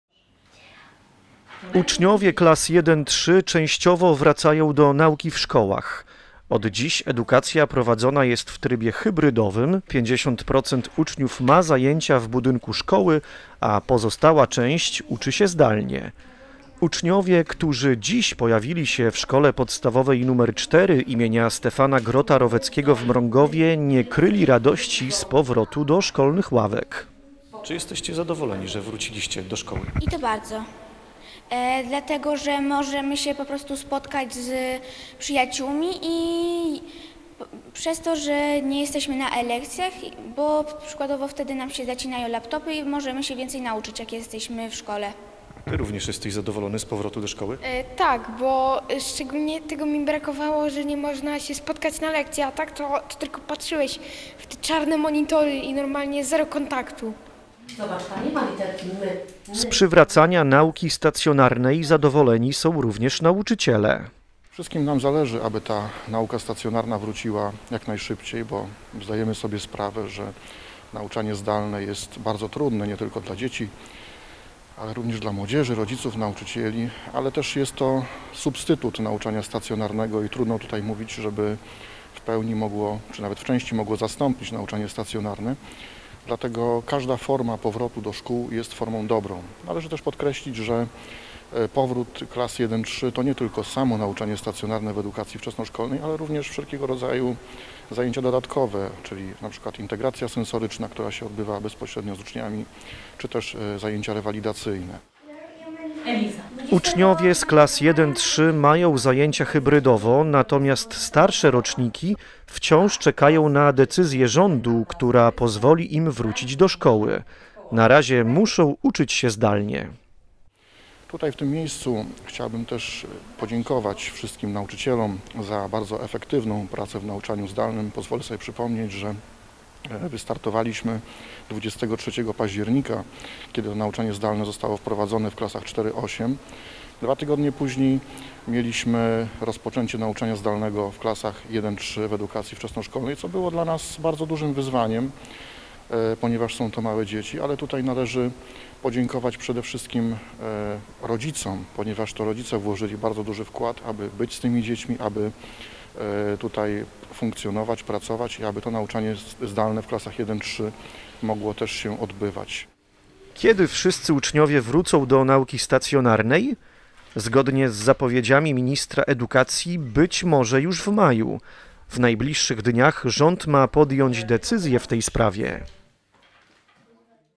Młodsi uczniowie wracają do szkół
Uczniowie klas I-III częściowo wracają do nauki w szkołach. Od dziś edukacja prowadzona jest w trybie hybrydowym - 50 proc. uczniów ma zajęcia w budynku szkoły, a pozostała część uczy się zdalnie.

Czy uczniowie są zadowoleni z powrotu do szkolnych ławek?
Co o tym sądzą nauczyciele?
Sprawdziliśmy to w Szkole Podstawowej Nr 4 im. Stefana "Grota" Roweckiego w Mrągowie.